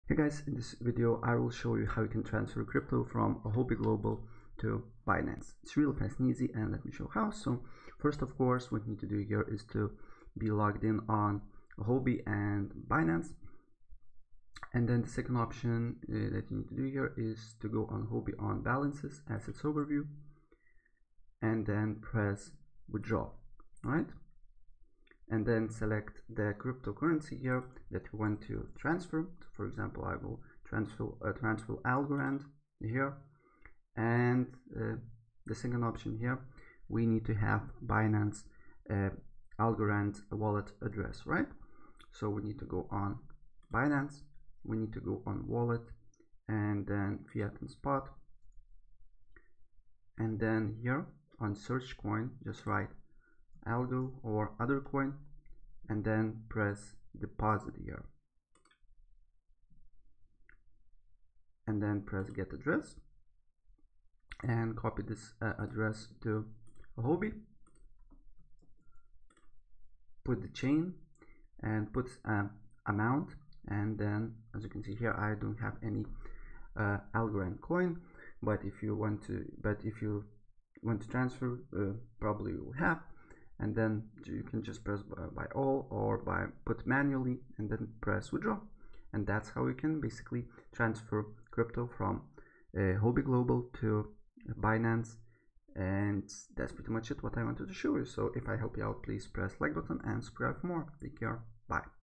How To Transfer Crypto From Huobi Global To Binance
In this video I will show you how to transfer crypto from huobi global to binance.  It's really easy and it will take you less than a minute to do so!